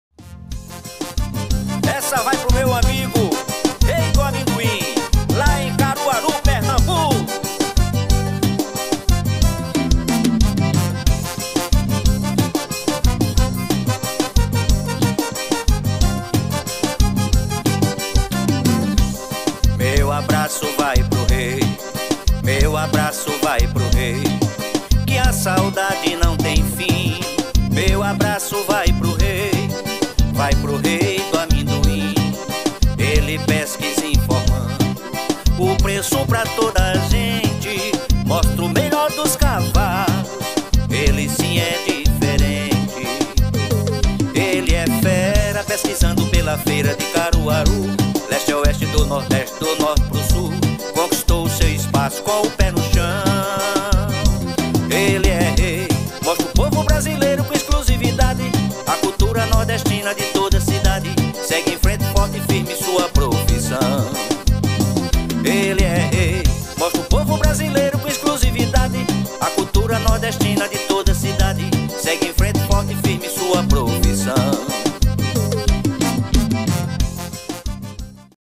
BOI CAINDO NA PISTA!! VAQUEJADA DO PARQUE FERNANDO LUCENA EM CARUARU PE!!
Canal de Paulo:

@Agrovida Paulo Brito